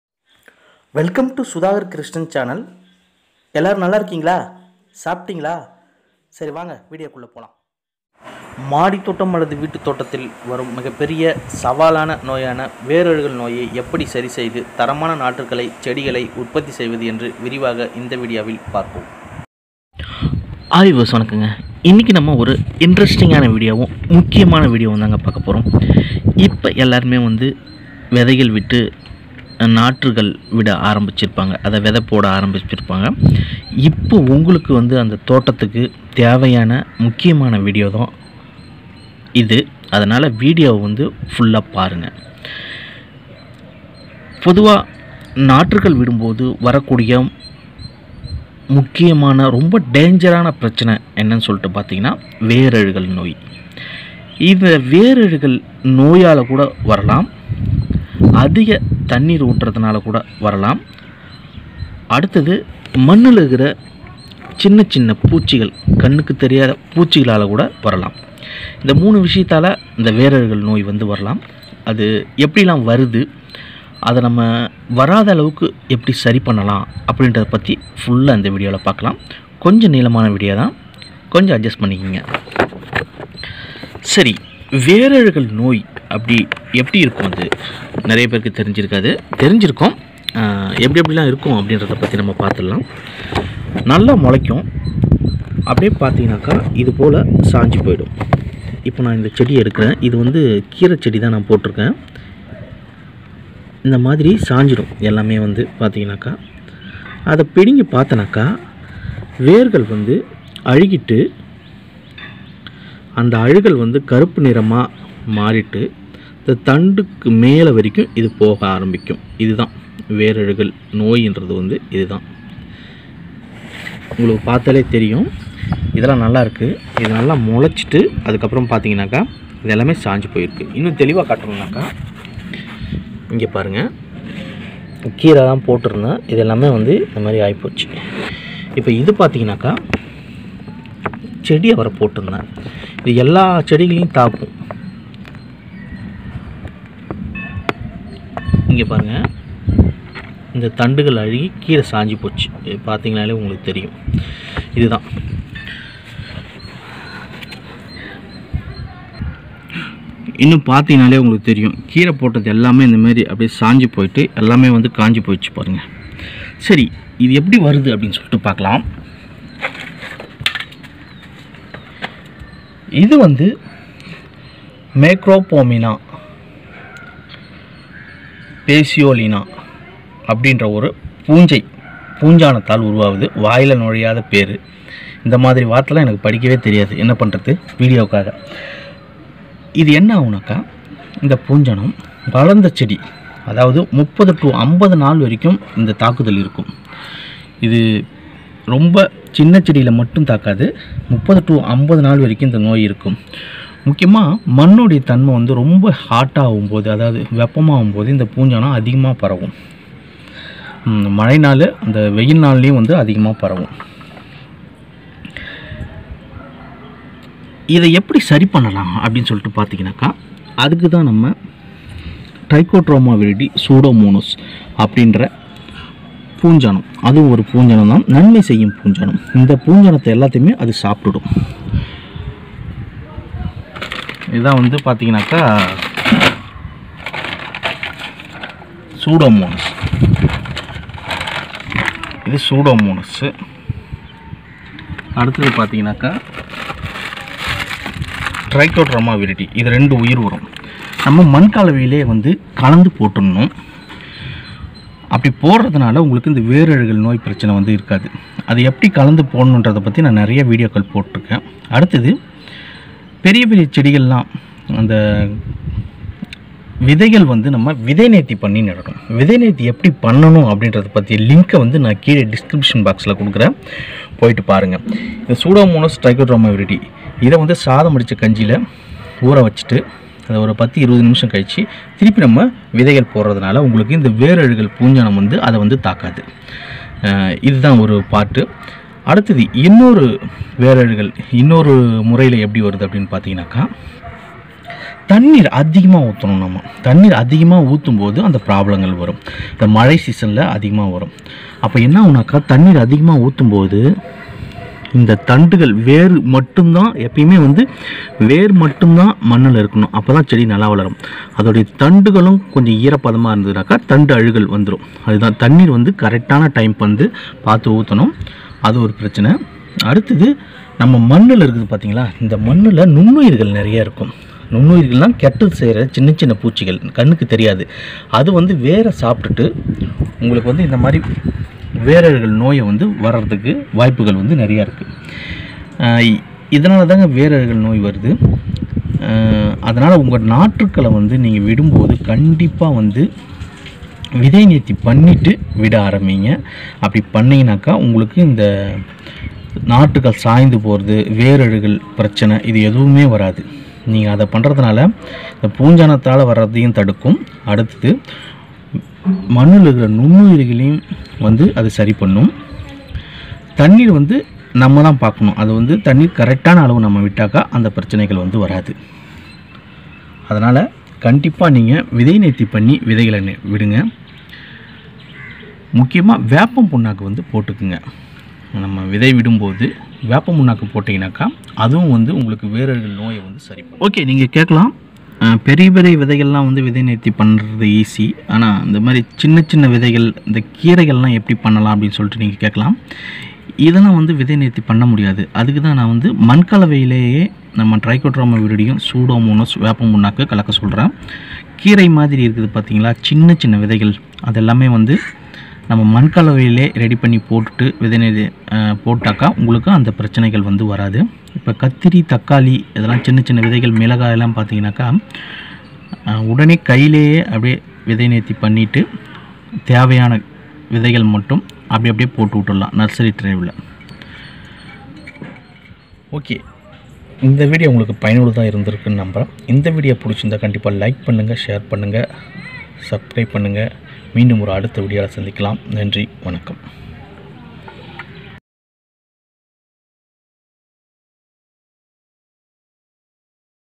உங்கள் தோட்டத்தில் நாற்றுகள் விடும் போது வரும் மிகப்பெரிய பிரச்சனைகள்! இந்த வீடியோவை மிஸ் பண்ணாதீங்க
sudagarkrishnan மாடித் தோட்டம் அல்லது வீட்டுத் தோட்டத்தில் செடி கொடிகளில் வரும் பயங்கர சவாலான விஷயமான வேர் அழுகல் நோயை எப்படி சரி செய்யலாம் என்று விரிவாக பார்ப்போம்.
விதை நேர்த்தி செய்வது எப்படி?